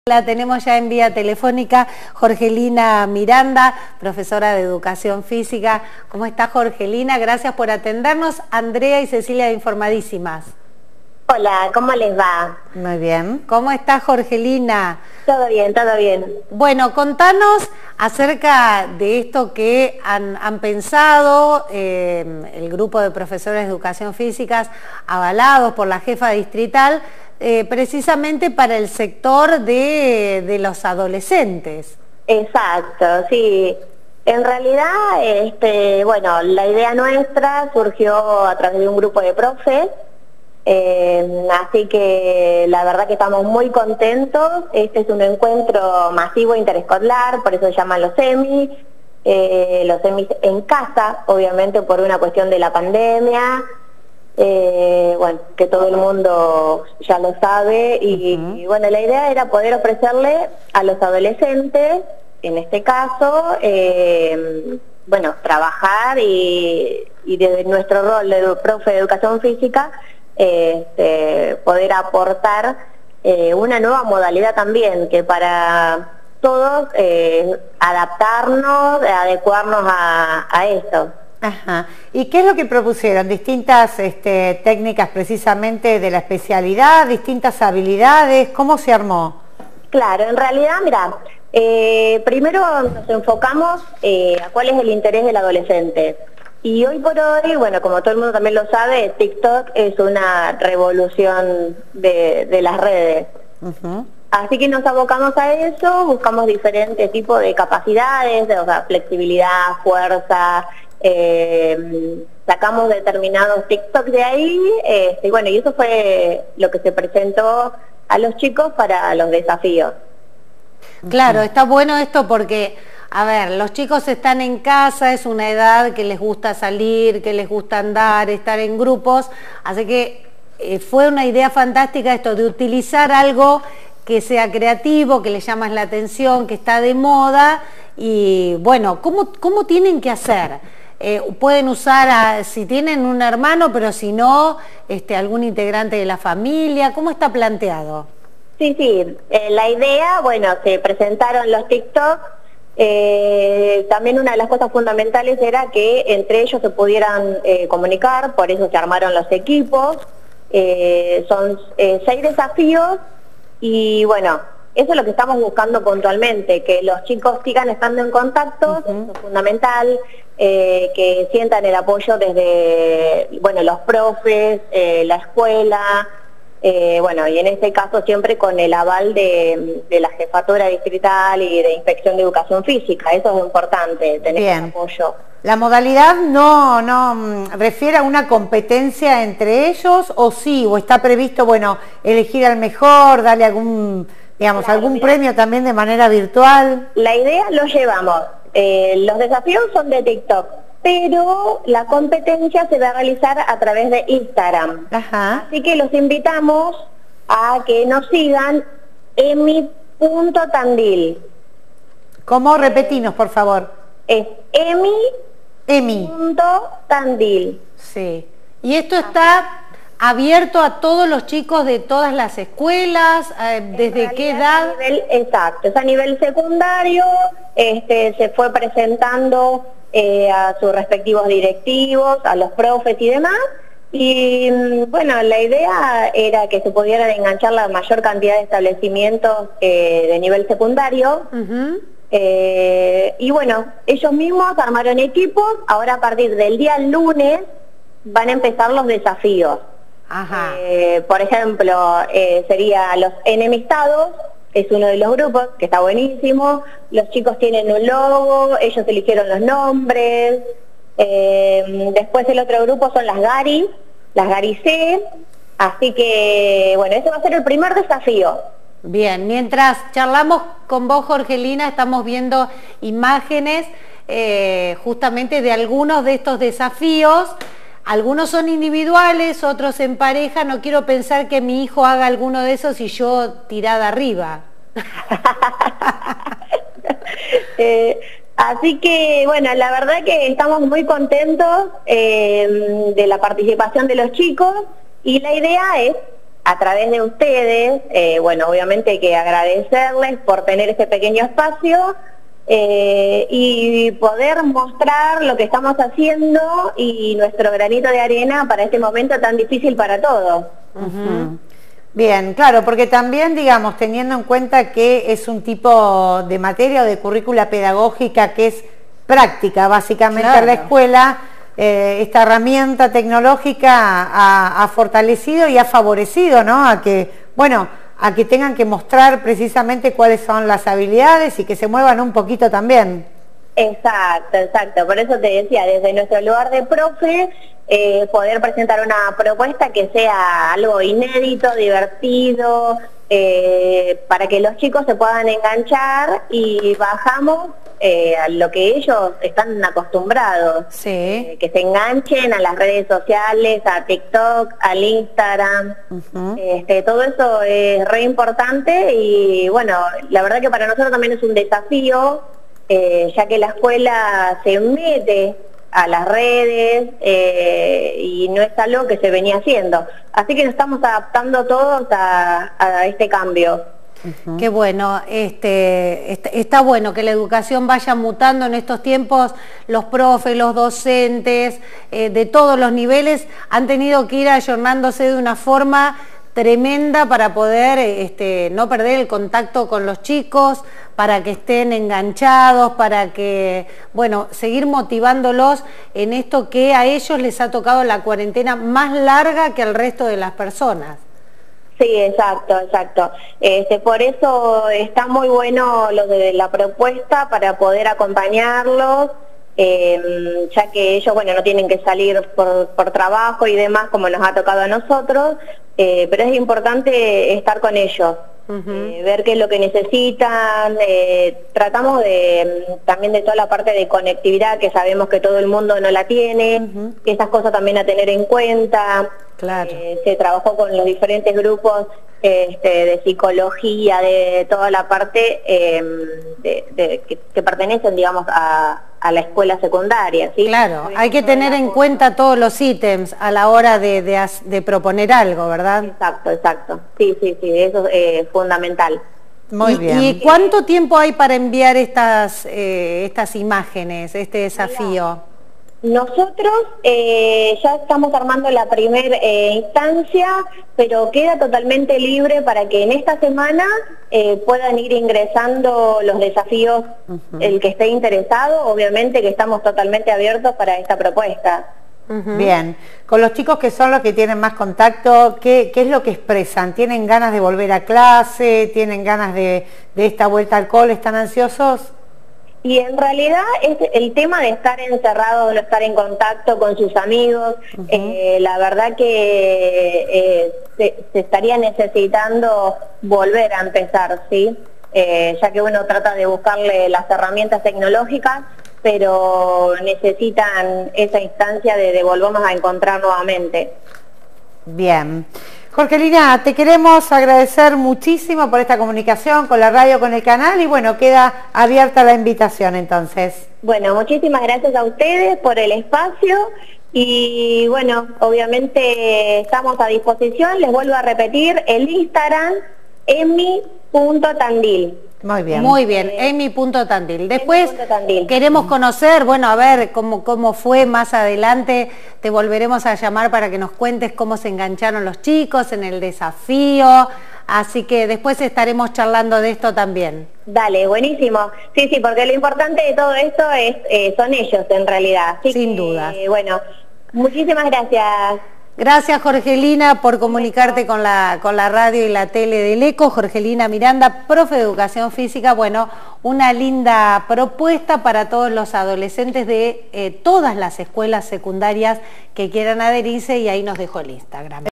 Con el aval de Jefatura Distrital, desafían a jóvenes de secundaria a participar de juegos virtuales
Jorgelina Miranda - Profesora de Educación Física, dialogó con Informadisimas por Eco TV y la 104.1 Tandil FM.